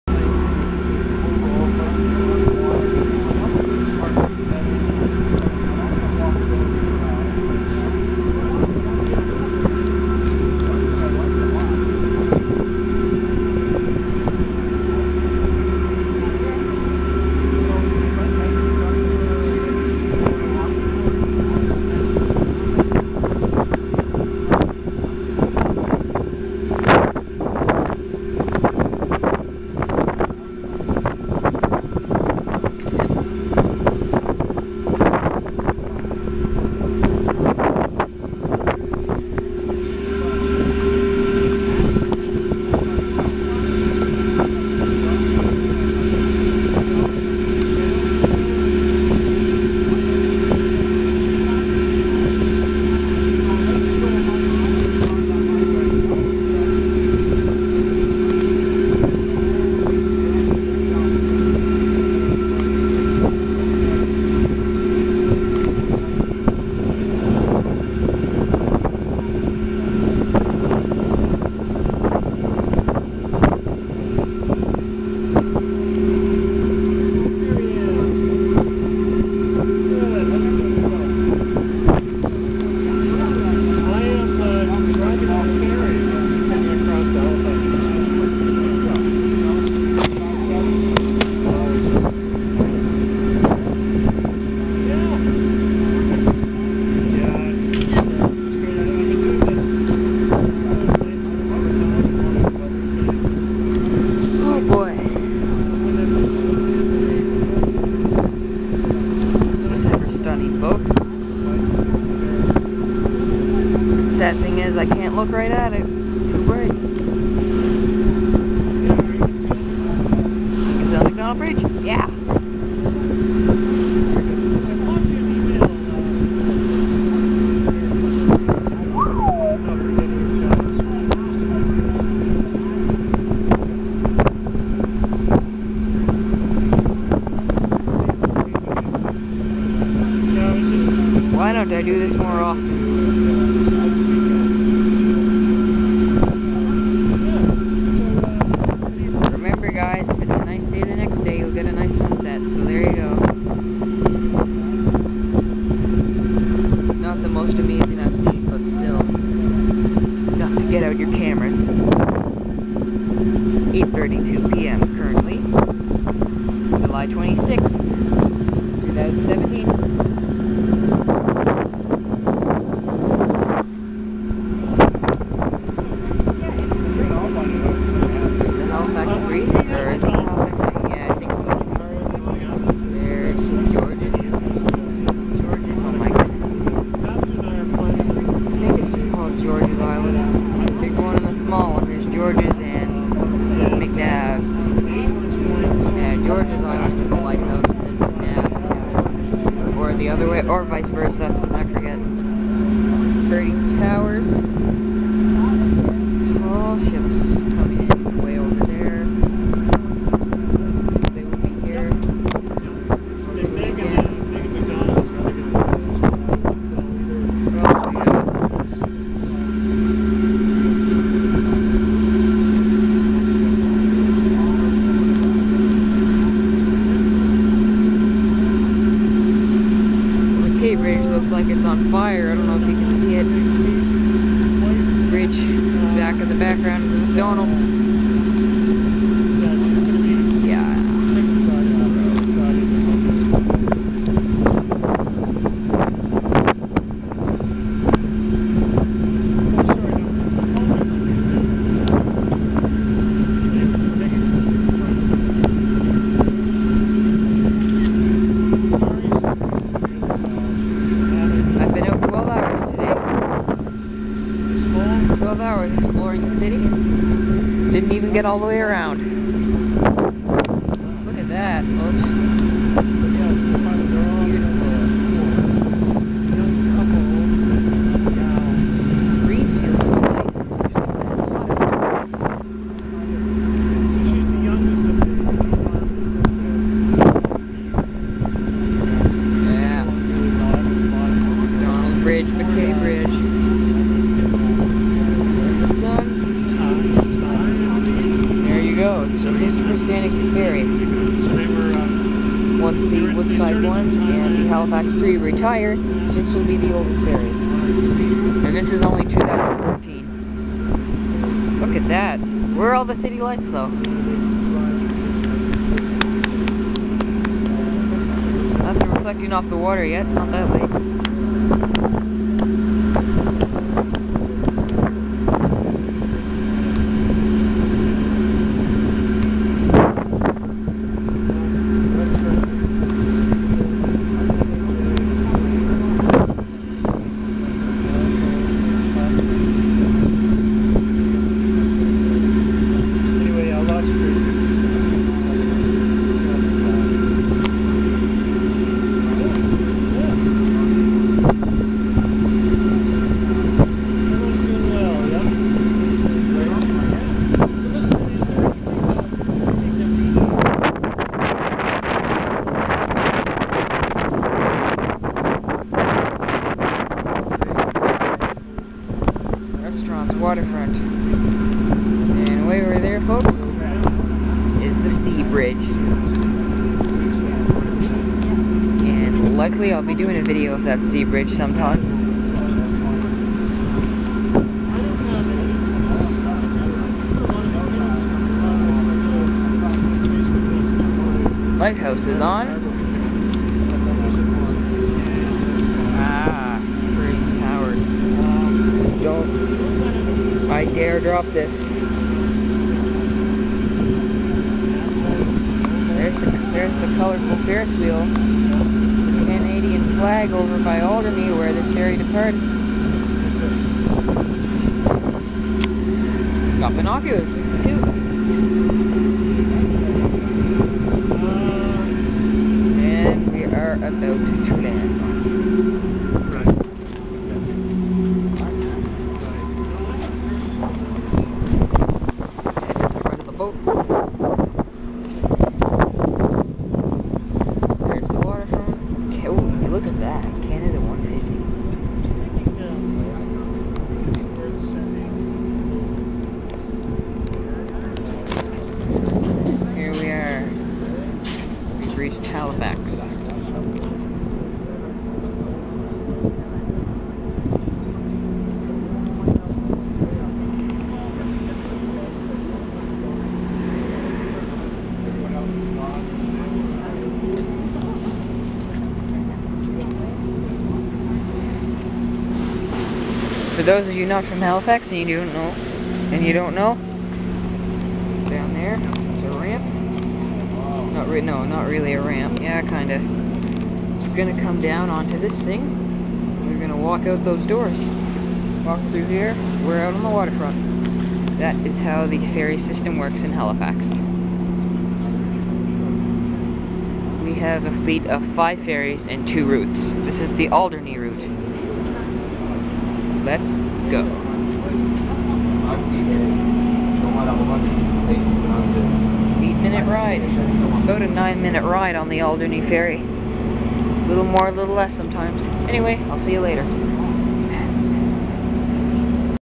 halifax transit: A Ride On The Ferry At Dusk
Enjoy the stunning view.